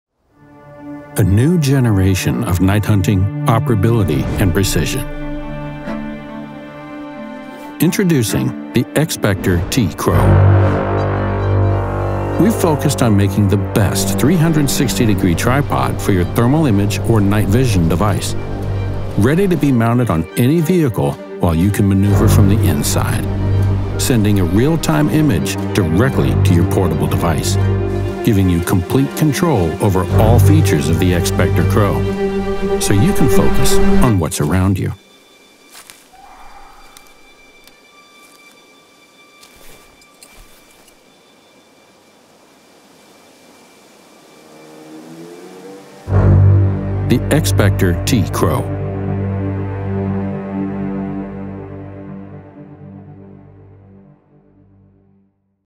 XSPECTER T CROW XR-II
Introducing the XSPECTER T-CROW
A new generation of night hunting.

Transform your thermal handheld-device or night-vision in a fully controllable 360 degree dome camera.
Use the wifi stream of your device to sending a real time image directly to your portable device.

exclusivly @frankonia.de